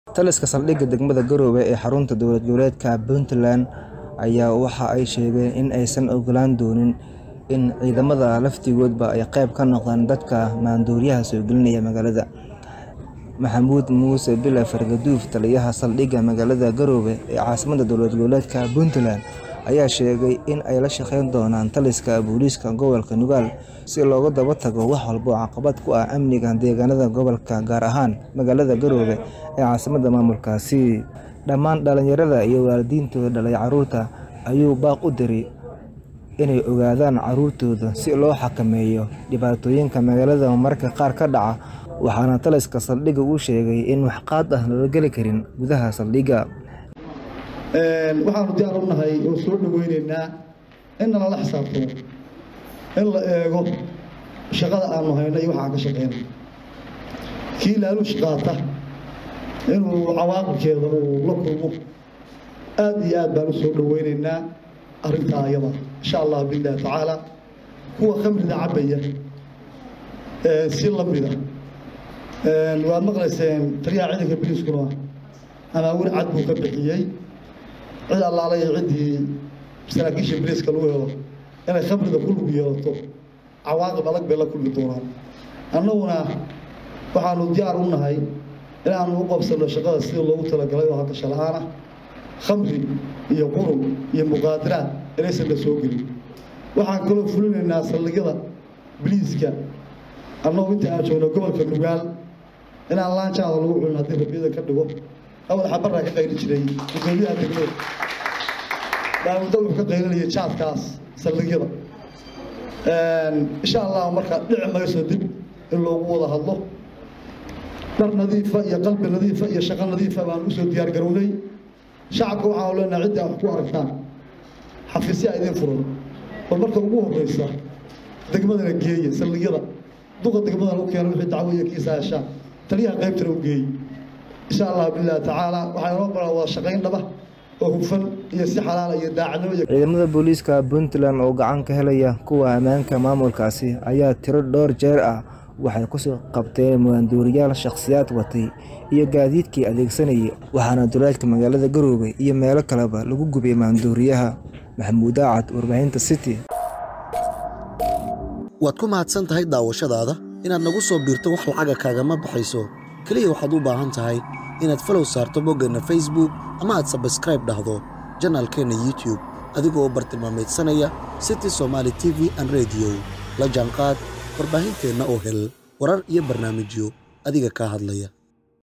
Taliska Booliiska Saldhiga Garoowe oo Waalidiin Magaalada ugu baaqay inay Xakameeyaan Caruurtooda
Taliska Booliiska Saldhiga Garoowe oo Waalidiin Magaalada ugu baaqay inay Xakameeyaan Caruurtooda Maxamuud Daacad ayaa Kawaramaya